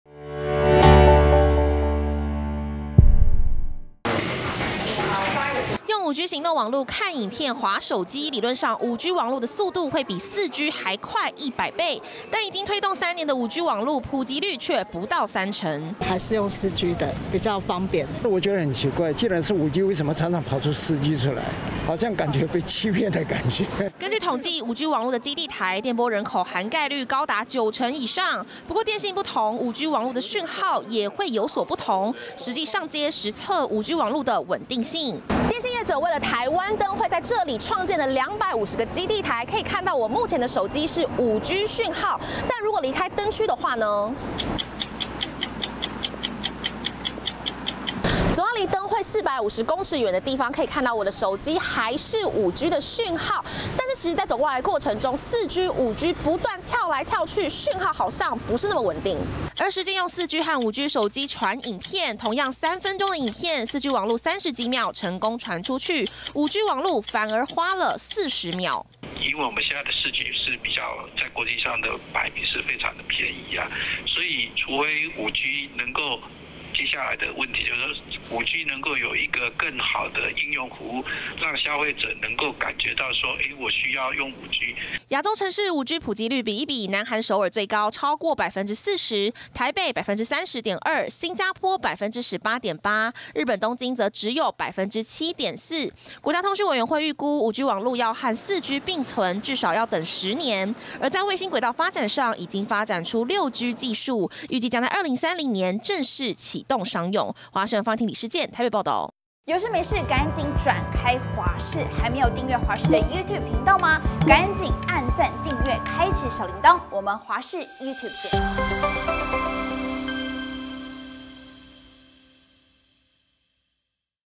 5G網路推動3年 全台普及率仍不到3成｜華視新聞 20230206
5G網路推動3年，但普及率卻不到3成。國家通訊委員會指出，主要和民眾認為，4G和5G使用差異不大，以及5G基地台基地台少，費率相對較高有關。實際走上街頭，5G行動網路似乎不太穩定，在室內裡面，5G行動網路更直接跳到4G，就連傳同樣的影片，4G行動網路都反而比5G快一些。